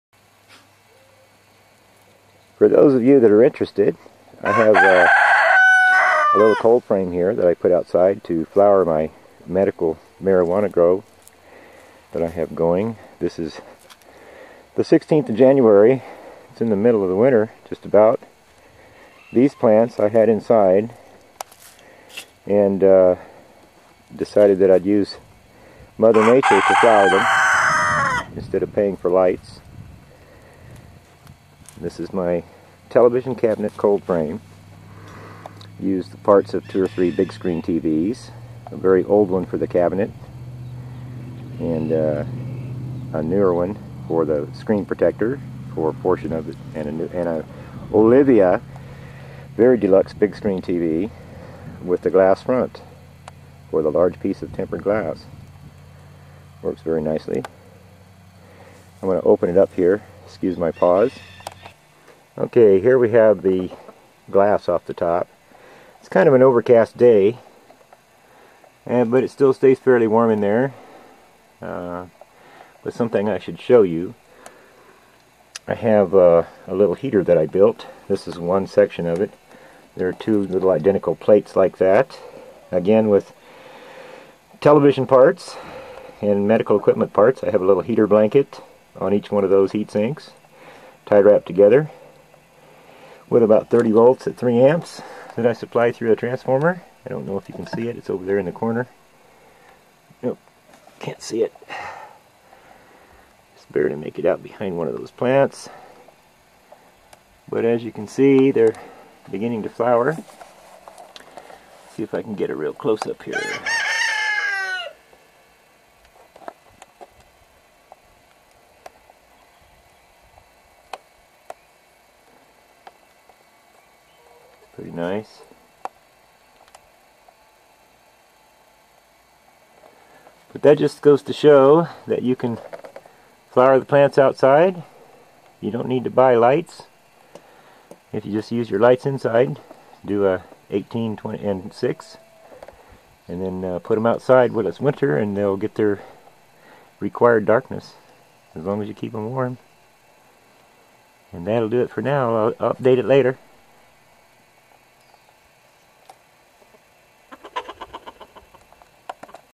Cannabis Flowering Outside in Winter
This video is the first of a short series I plan to do to document my attempt to induce flowering and fruiting in my first medical marijuana grow. I used "undocumented" bag seeds of unknown origin and age so I didn't have the expense of buying seeds of established strains in case I experienced failure in my novice enterprise. I planted the seeds in the first week of October and kept them at 18/6 until the week of Christmas, when I constructed the coldframe from old TV cabinet parts. I have clones of some of the plants so I can continue the strain in case I happened to get something very good out of the first grow cycle. I basically have been building and adapting ideas from others on YouTube to develop my growing and propagating setup without spending a fortune on materials to put it all together. I veg'ed inside with flourescents(160W, 6500K) and a thrown-together heater, surrounded by styrofoam panels faced with mylar from the Hydro shop, watered by hand, in soil and vermiculite, fed regularly with mild mixtures of hydroponic nutes and fish emulsion. When I put them into the coldframe, I started feeding them rose food for the high P and K. I plan to flush them with plain water a couple of weeks before I harvest. Then I have to learn about drying and curing!